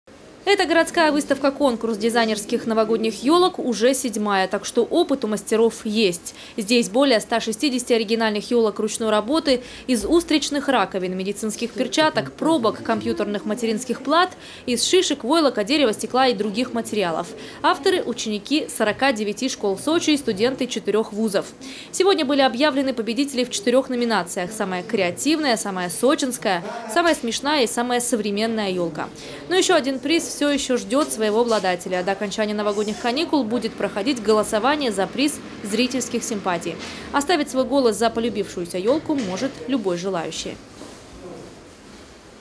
В Сочи открылась выставка дизайнерских елок. Новости Эфкате
Вернисаж развернулся в Доме моды.

Эта городская выставка-конкурс дизайнерских новогодних елок — уже седьмая. Так что опыт у мастеров есть. Здесь более 160 оригинальных елок ручной работы: из устричных раковин, медицинских перчаток, пробок, компьютерных материнских плат, из шишек, войлока, дерева, стекла и других материалов. Авторы — ученики 49-ти школ Сочи и студенты 4 ВУЗов. Сегодня были объявлены победители в 4 номинациях: самая креативная, самая сочинская, самая смешная и самая современная елка. Но ещё один приз все ещё ждёт своего обладателя. До окончания новогодних каникул будет проходить голосование за приз зрительских симпатий. Оставить свой голос за полюбившуюся ёлку может любой желающий.

Сочи. Новости.
Телеканал Эфкате.